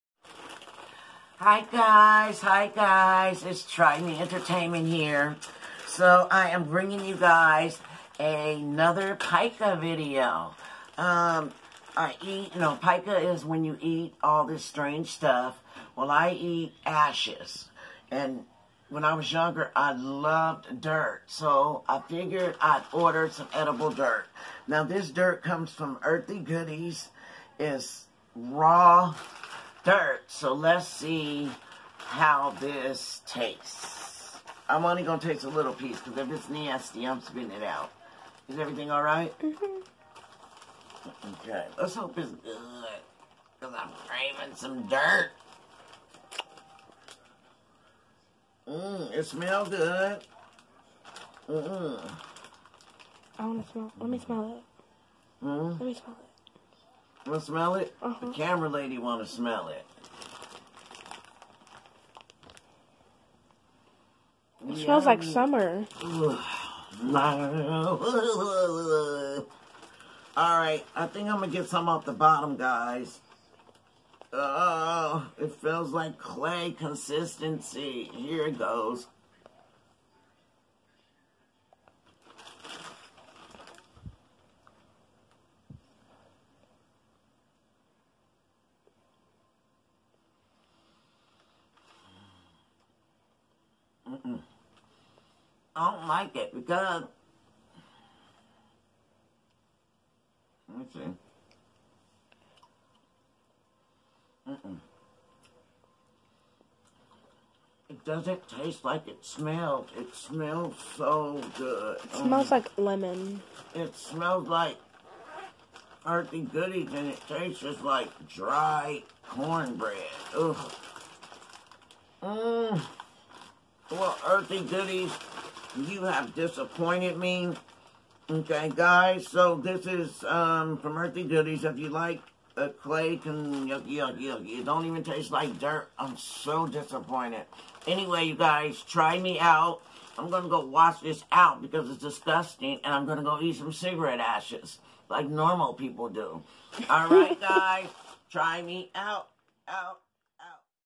(PIKA) TRYING EDIBLE DIRT DON'T KNOW WHY I HAVE THIS CONDITION I JUST DO!!
HEY FAM, SO I PURCHASED SOME EDIBLE DIRT FROM EARTHLY GOODIES. THEY SMELLED REALLY GOOD LIKE WET DIRT, BUT NO WAY YUCK TASTE LIKE CLAY and SAND. I AONT GET THIS KIND AGAIN.